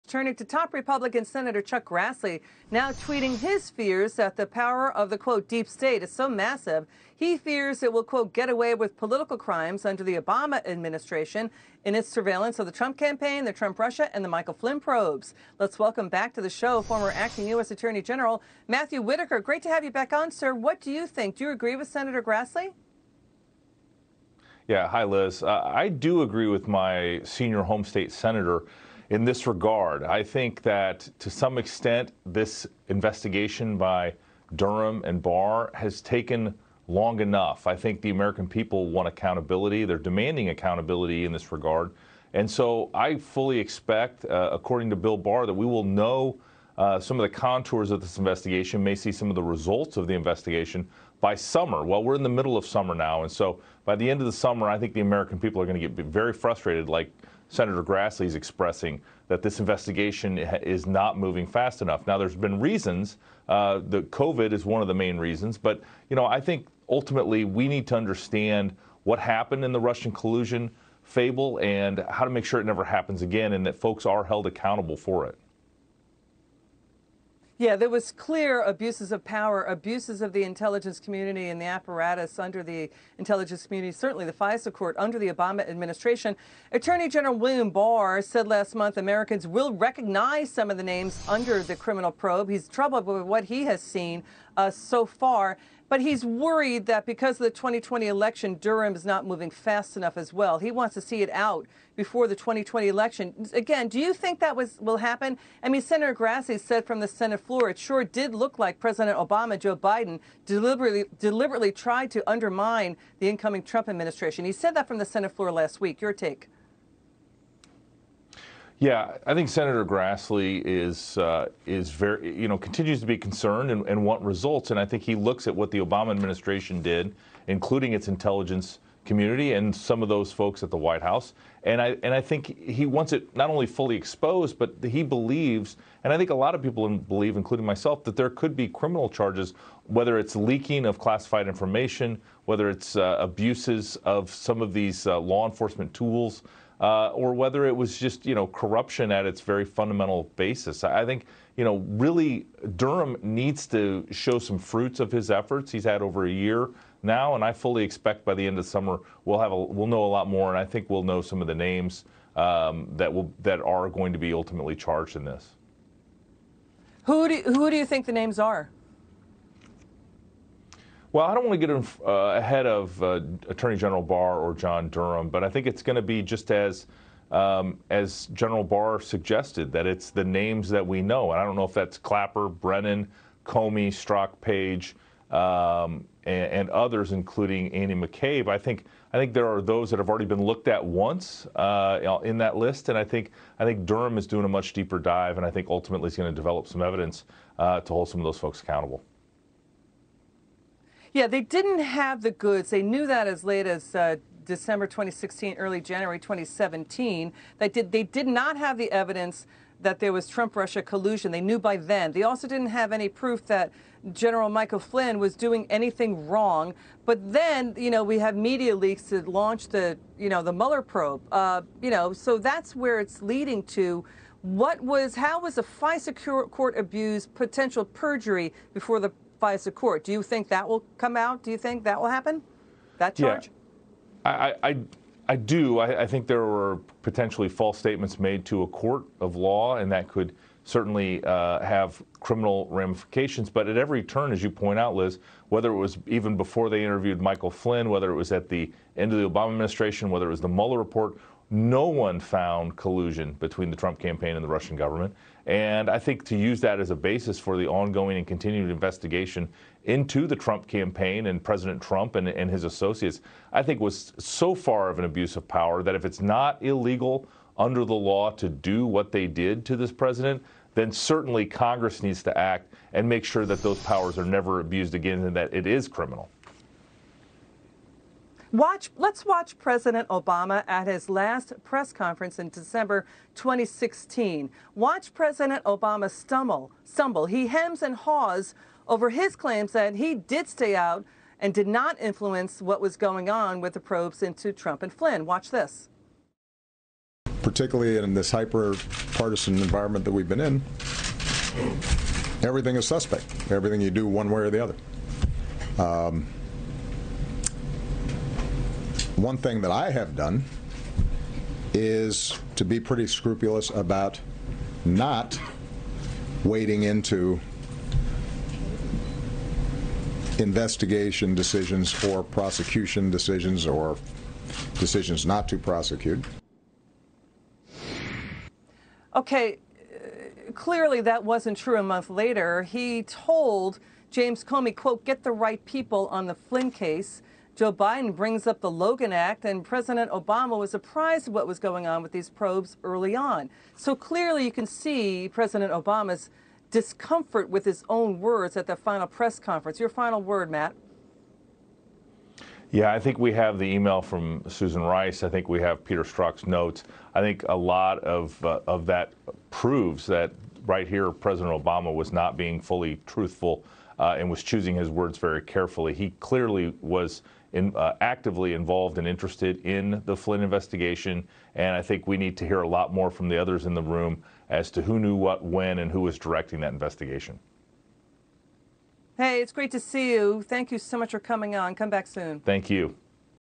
Matthew Whitaker claims Obama was 'clearly' involved in Flynn investigation
Former Acting U.S. Attorney General Matthew Whitaker tells FOX Business’ Elizabeth MacDonald that the American people ‘want accountability’ because the Russia-probe investigation ‘is not moving fast enough.’